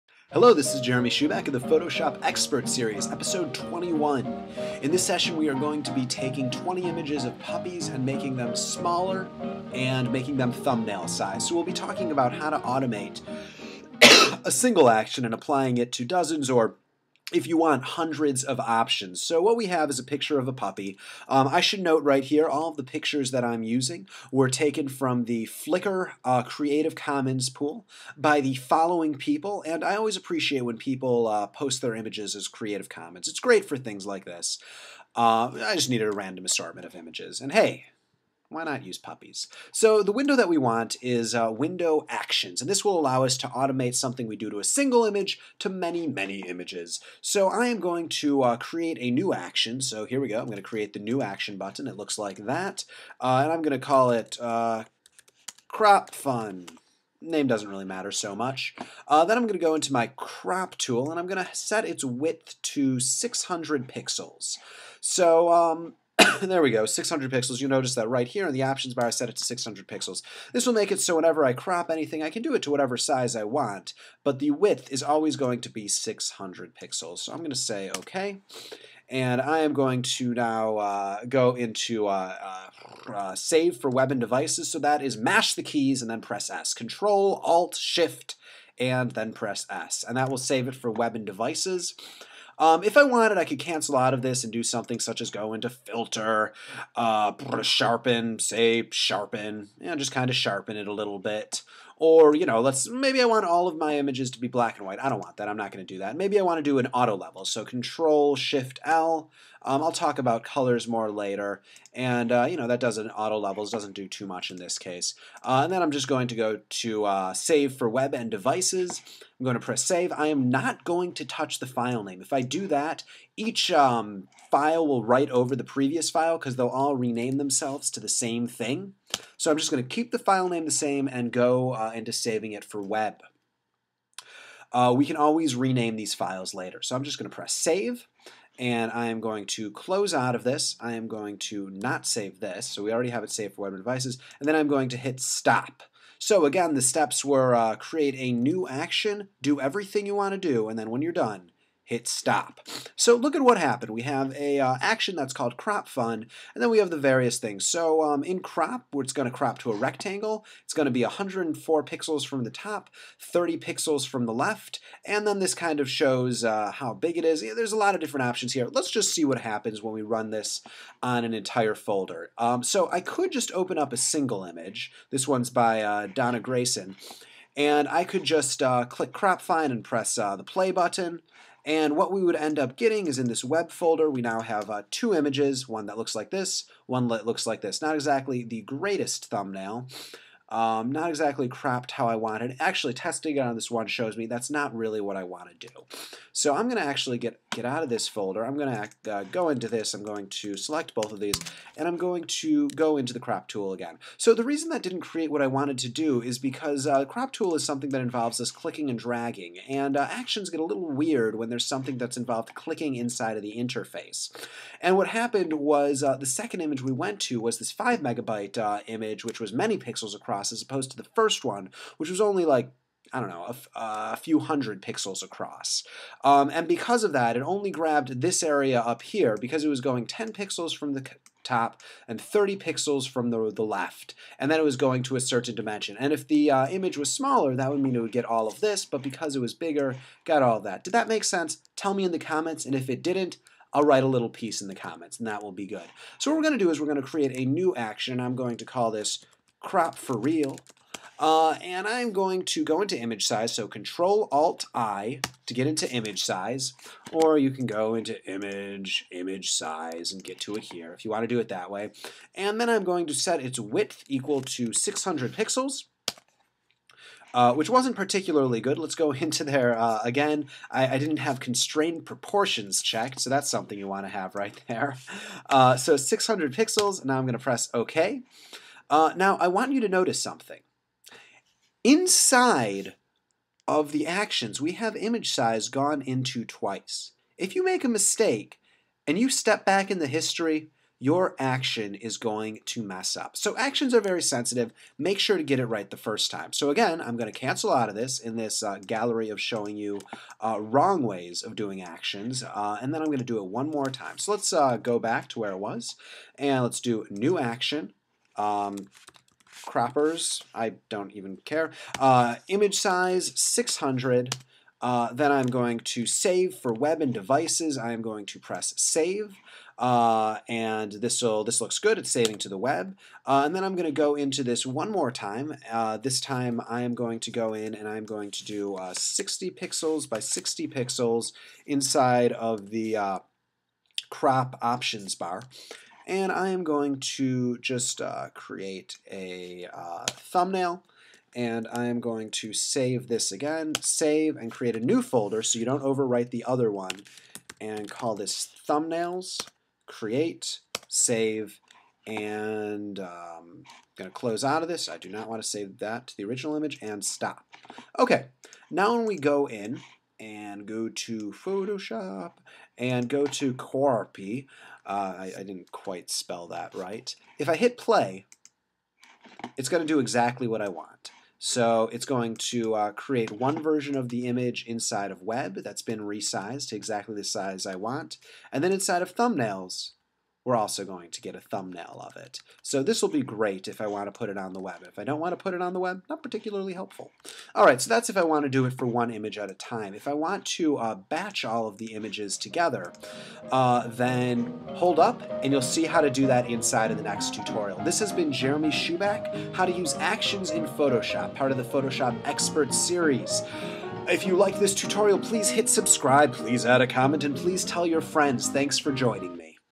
Using Actions in Photoshop
How to automate monotonous tasks such as resizing 100's of images, saving out multiple images for the web, or do just about anything again and again, repeatedly. How to create a bunch of thumbnails without needing to go into each one individually.